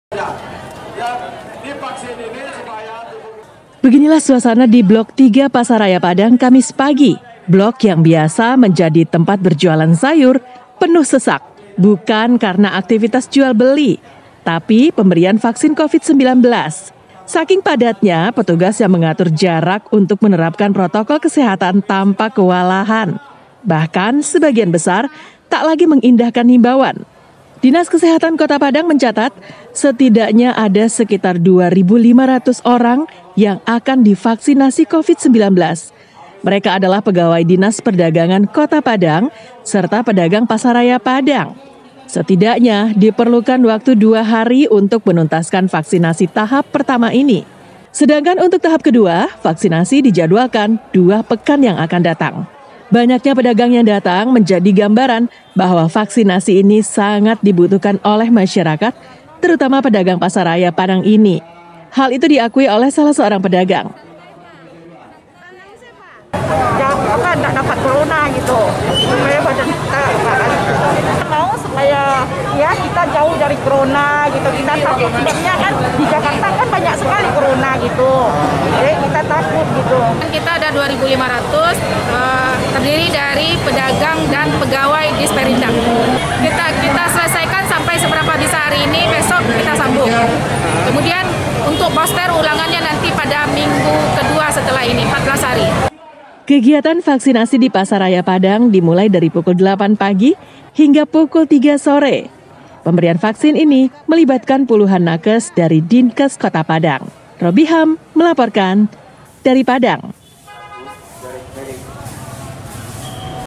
PEDAGANG PASAR RAYA PADANG ANTUSIAS IKUTI VAKSINASI
PELAKSANAAN VAKSINASI BAGI PEDAGANG DI PASAR RAYA PADANG